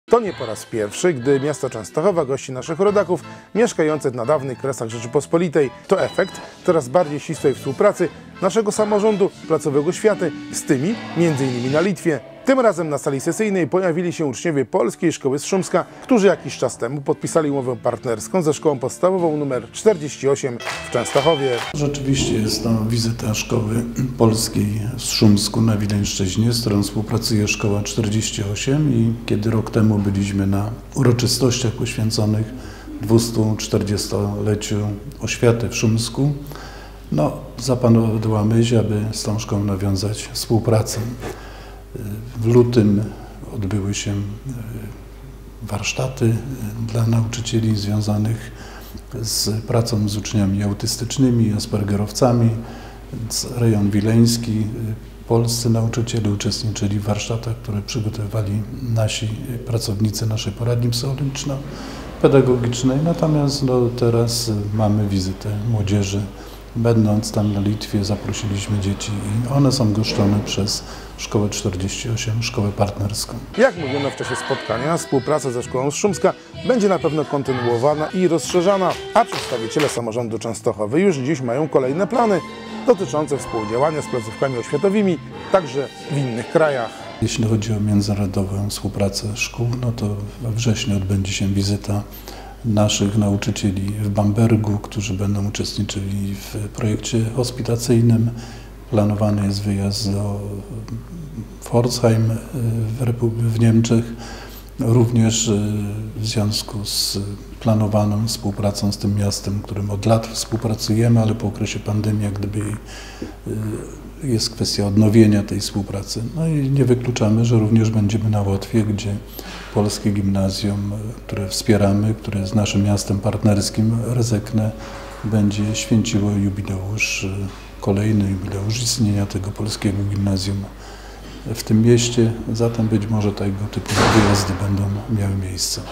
Uczniowie z Szumska na spotkaniu z władzami Częstochowy
Uczniowie polskiej szkoły z Szumska na Litwie spotkali się z przedstawicielami częstochowskiego samorządu.